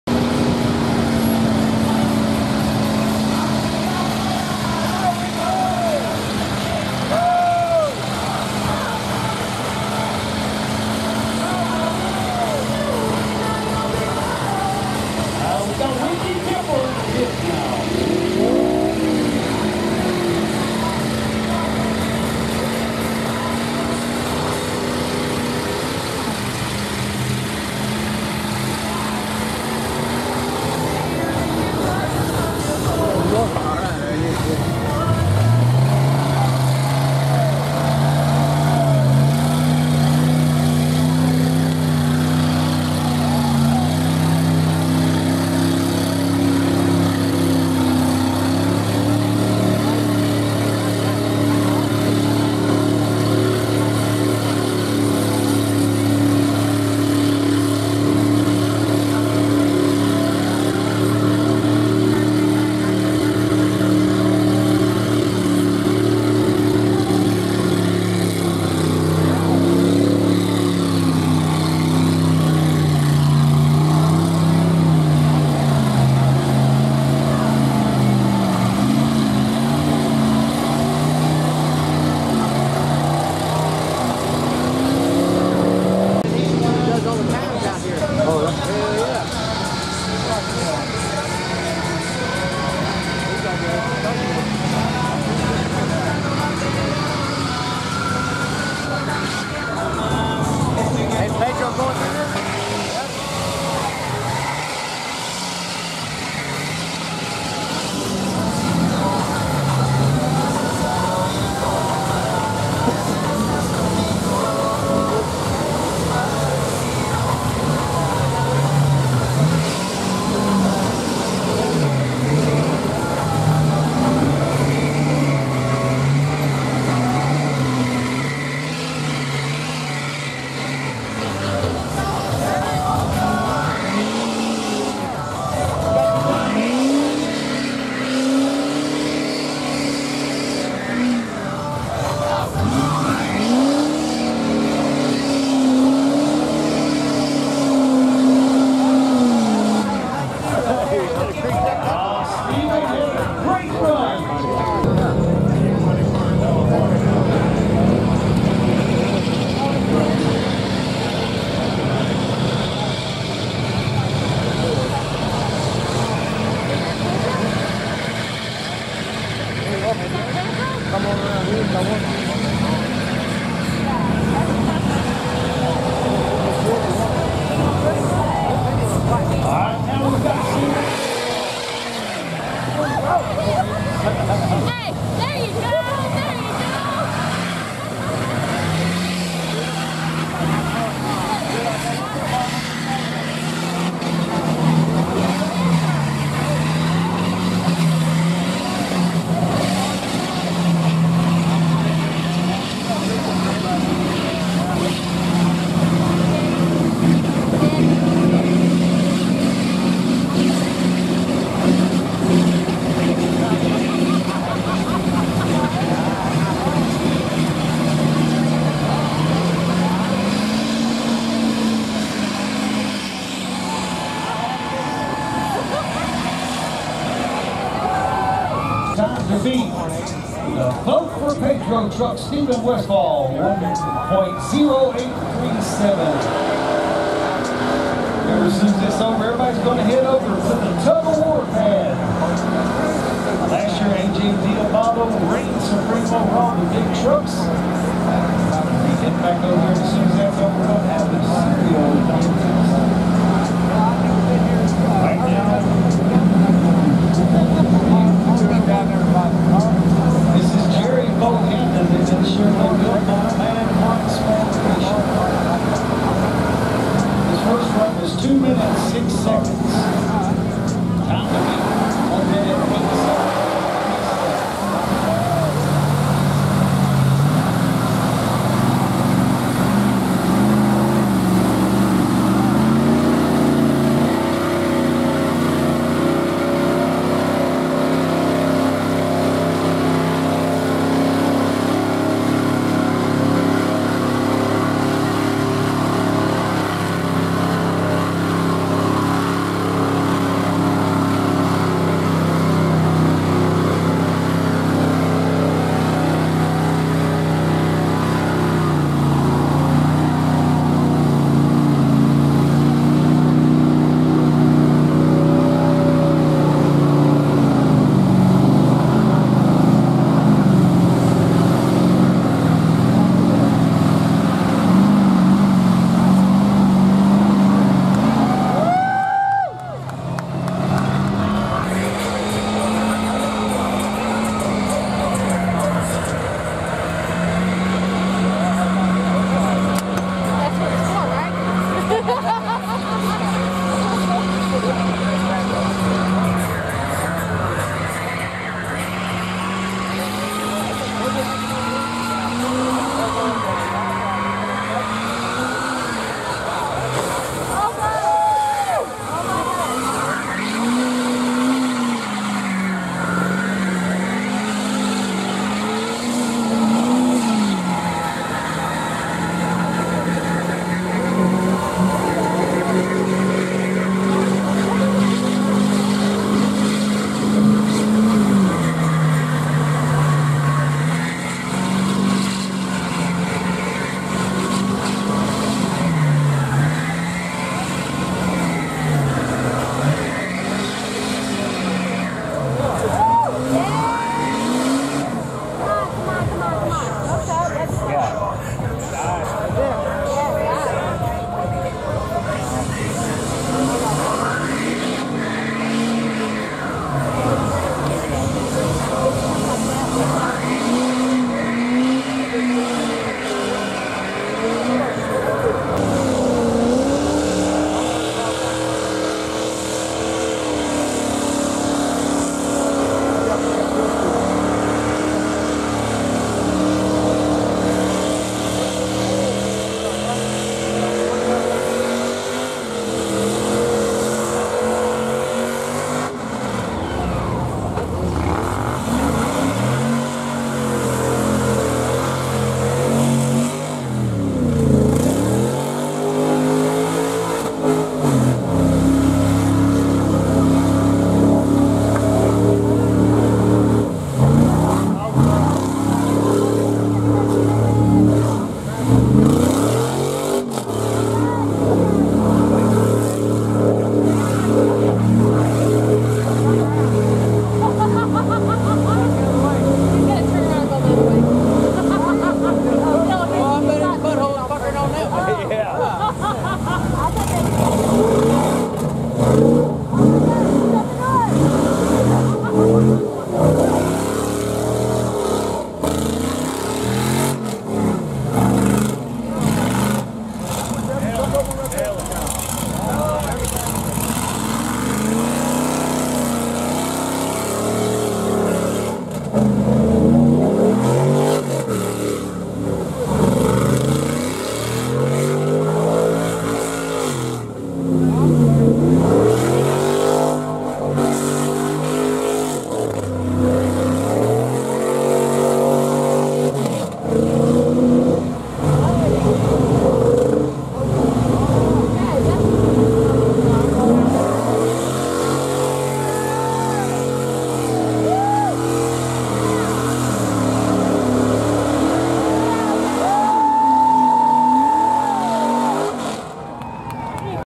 2021 Fall Mud Crawl Bounty Hole
Rednecks with Paychecks Fall Mud Crawl final round of the bounty hole.